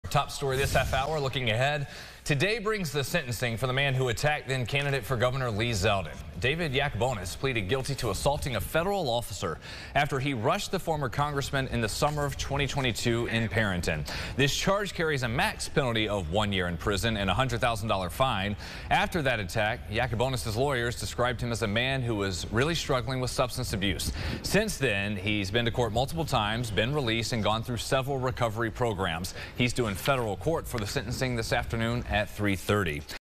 Zeldin attacker David Jakunonis to be sentenced Thursday
The man who pleaded guilty to trying to attack former Congressman Lee Zeldin will be sentenced Thursday morning.